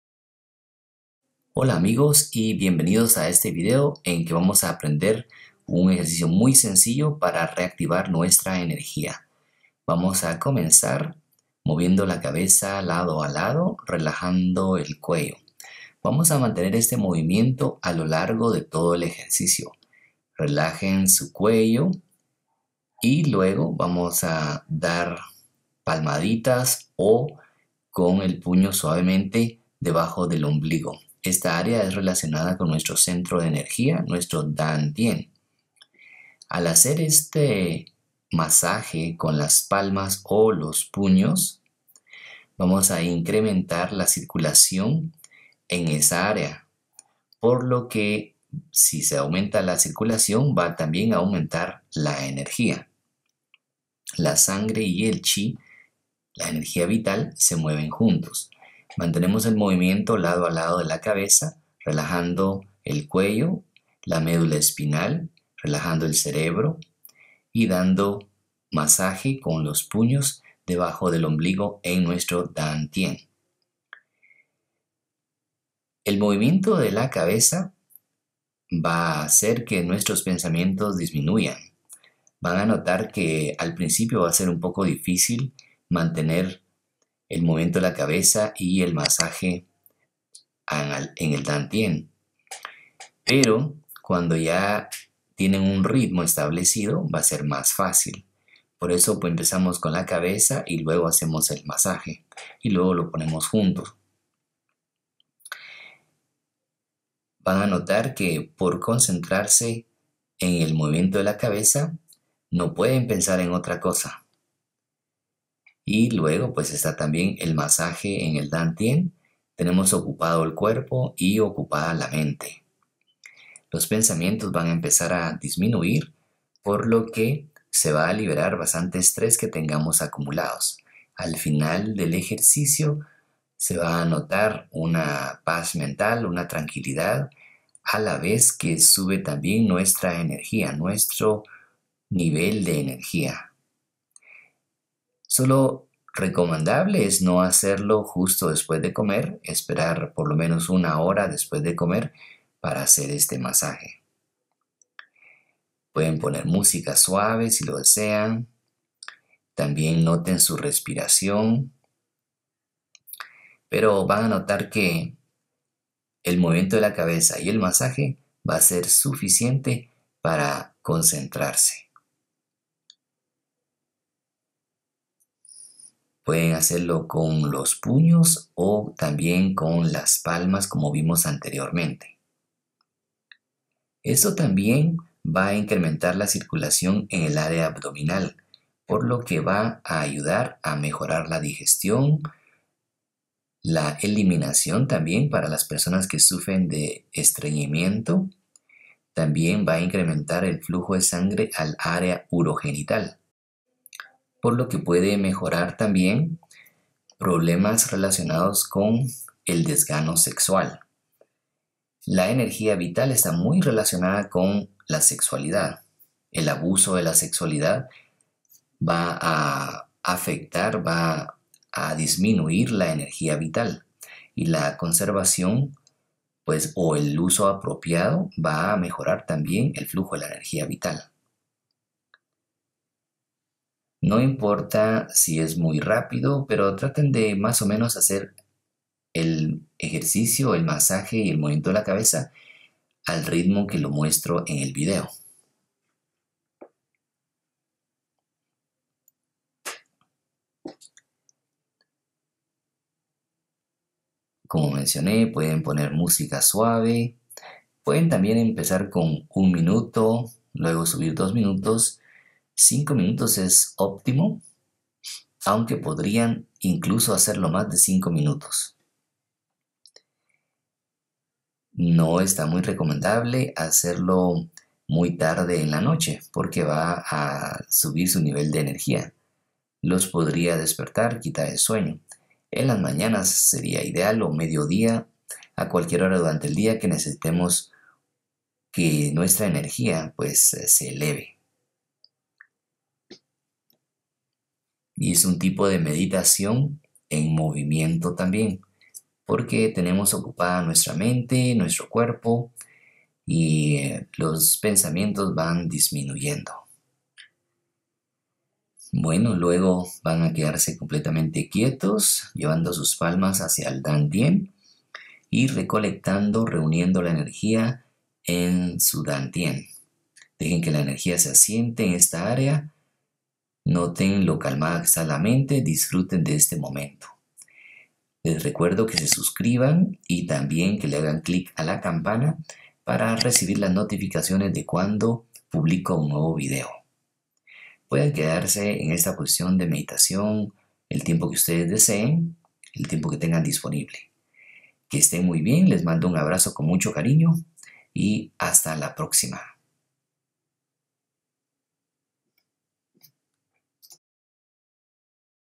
Eleva tu ENERGÍA con este sencillo ejercicio
El Dan Tien, es nuestro centro de energía ubicado debajo del ombligo y en el centro del cuerpo.  Almacena nuestra energía vital (Chi).

Este masaje en el Dan Tiene junto con el movimiento de la cabeza incrementara el flujo de sangre en esa área y cámara la mente.

Cuando hay más flujo de sangre hay más flujo de energía (Chi), por lo que te sentirás con más energía y tu mente se calmara.

Un abrazo!